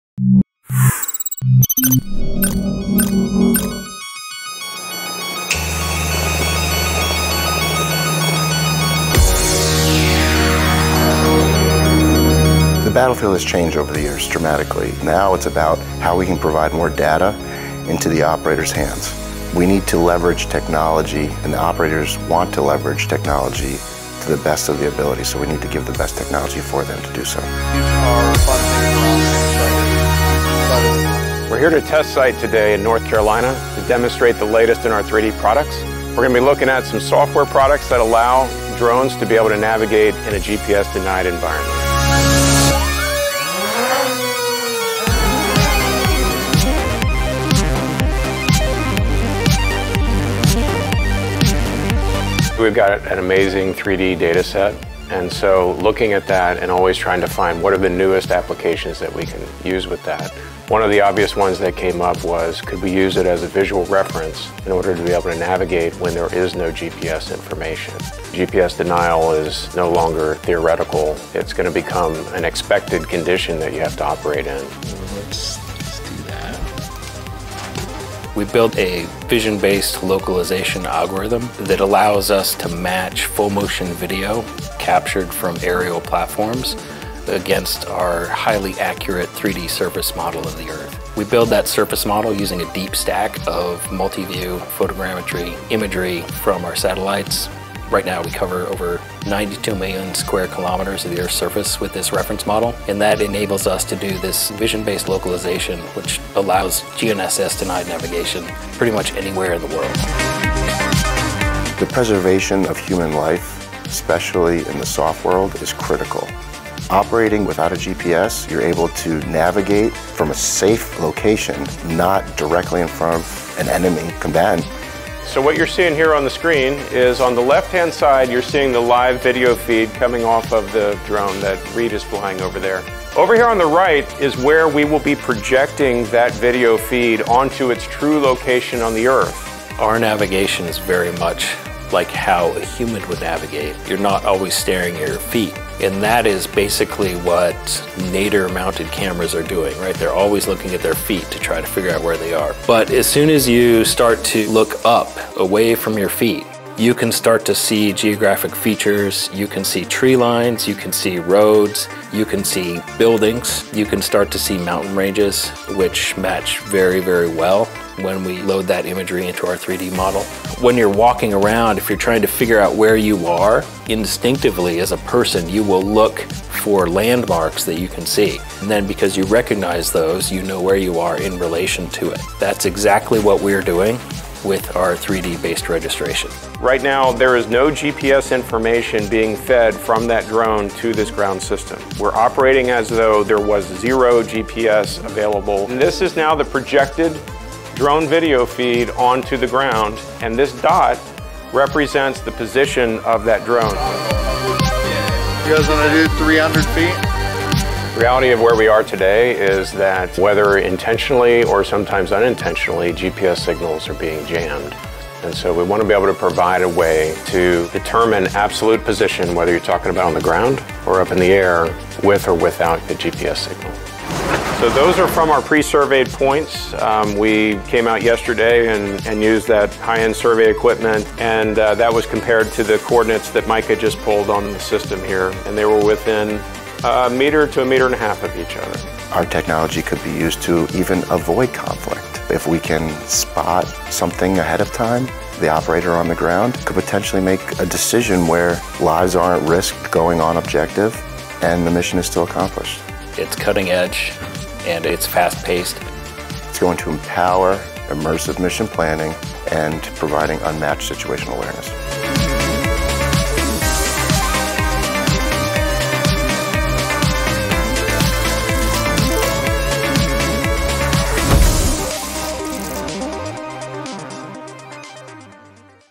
GPS-Denied Navigation Anywhere in the World
How do drones navigate without GPS or extra hardware?

Raptor, a suite of vision-based software products that use Vantor’s unique global 3D data, delivers a breakthrough terrain-based positioning system for drone navigation and sensemaking in GPS-denied environments. Discover how drones can navigate, position themselves and extract CAT-1 ground coordinates in real-time—using only their onboard camera and our global 3D terrain data. No GPS. No additional hardware.

🔹 How Raptor's vision-based system is changing autonomous ops
🔹 Why human-like navigation matters for real-world missions
🔹 How Raptor can empower operators—and even help avoid conflict.

Raptor was designed and developed to support both commercial and military missions.